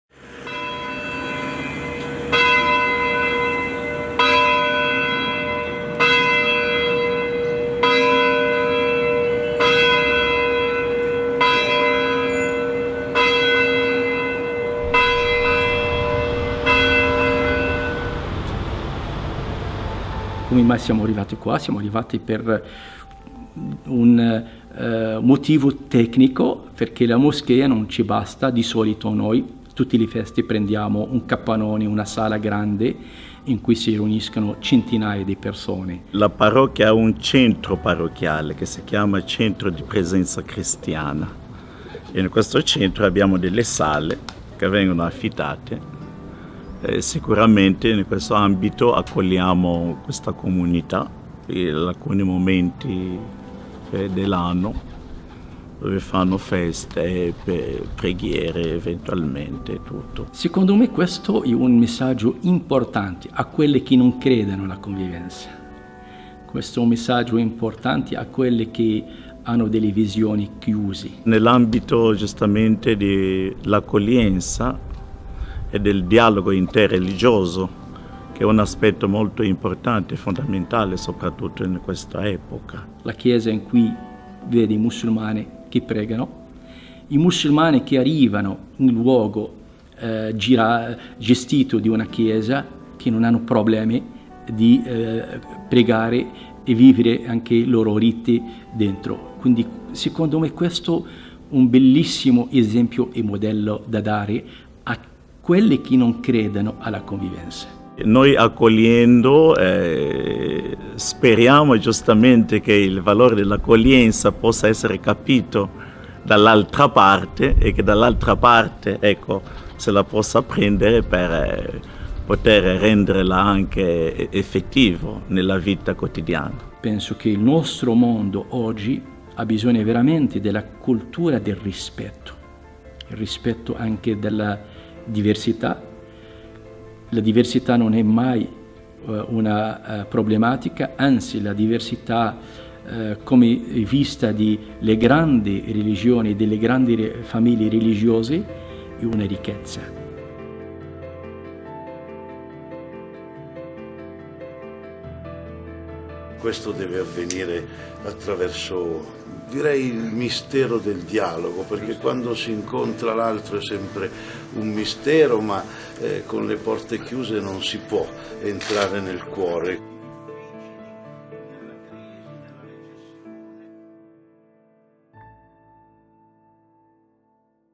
Religioni sotto lo stesso tetto - RSI Info
La comunità musulmana ticinese prega in uno spazio della chiesa cattolica di Pregassona. Il parroco: "Insegniamo l'accoglienza"
I rintocchi delle campane della chiesa risuonano alle 7.00 del mattino in via Terzerina, a Pregassona. È martedì 28 agosto, giorno della ‘id al-adha, la festa islamica del Sacrificio. Centinaia di membri della comunità musulmana del Ticino si dirigono verso la sala adiacente alla chiesa per andare a pregare. Il Centro di presenza cristiana – così si chiama lo spazio multiuso messo a disposizione dalla parrocchia – è stato allestito con i tradizionali tappeti per la preghiera islamica.
Pubblicato il: 28 settembre 2018, 05:54